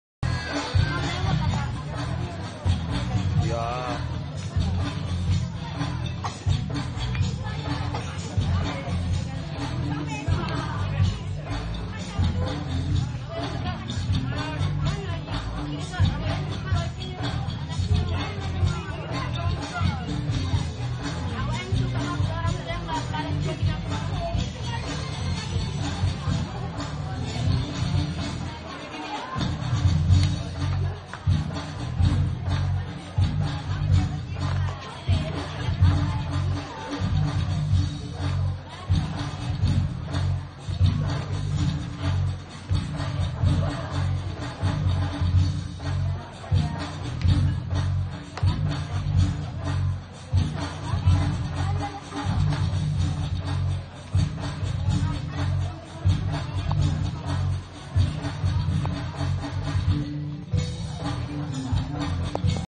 tajusalam besilam
pesantren modren tajusalam di besilam dalam rangka ulang tahun pesantren tajusalam